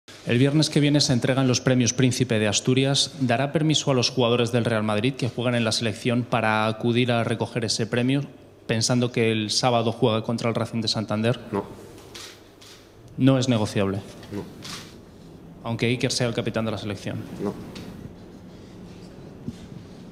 Los internacionales españoles del Madrid no recogerán el Príncipe de Asturias
José Mourinho no permitirá a los cinco futbolistas del Real Madrid que ganaron el Mundial de Sudáfrica (Iker Casillas, Sergio Ramos, Raúl Albiol, Álvaro Arbeloa y Xabi Alonso) que viajen a Oviedo el viernes a recoger el premio Príncipe de Asturias de los deportes. Al día siguiente juegan contra el Racing de Santander.